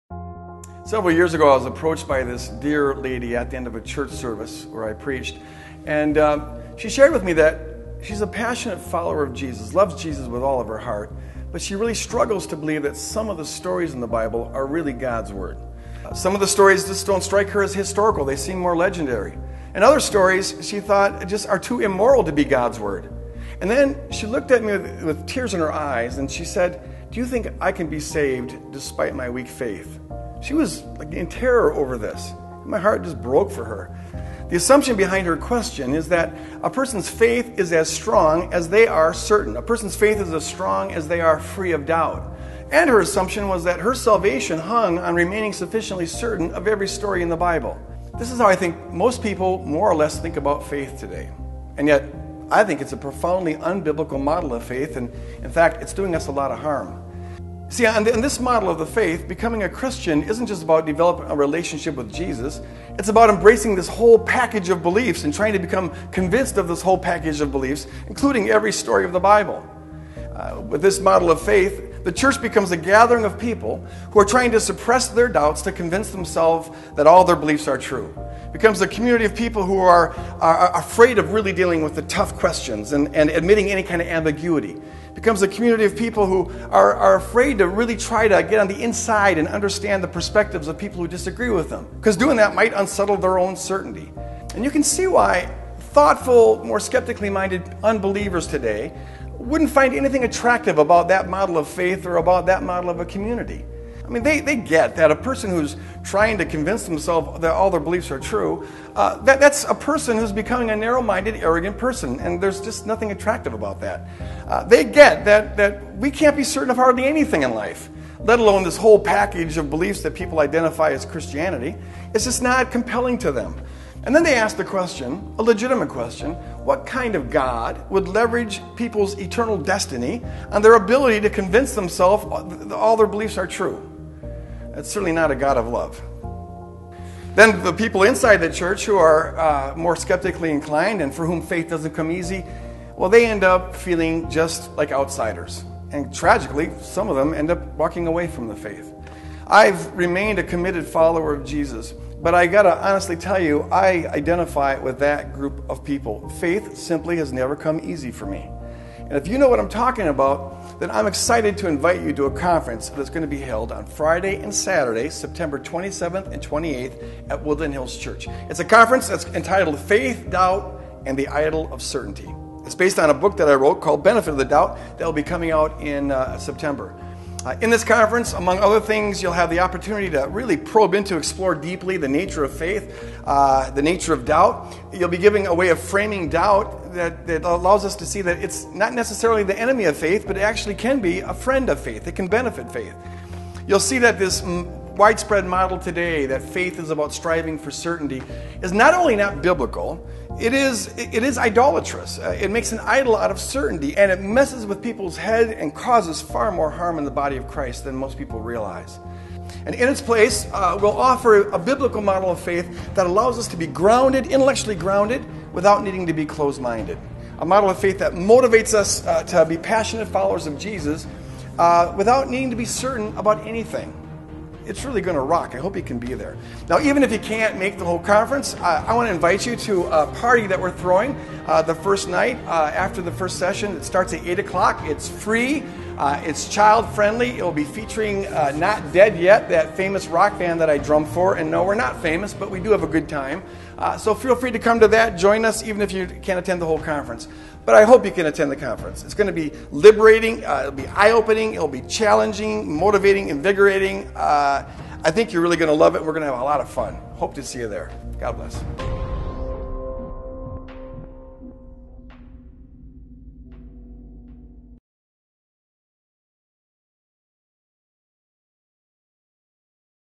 Faith, Doubt and the Idol of Certainty Conference - Reknew.org
The Faith, Doubt and the Idol of Certainty Conference will be held at Woodland Hills Church in St. Paul, MN on September 27th and 28th.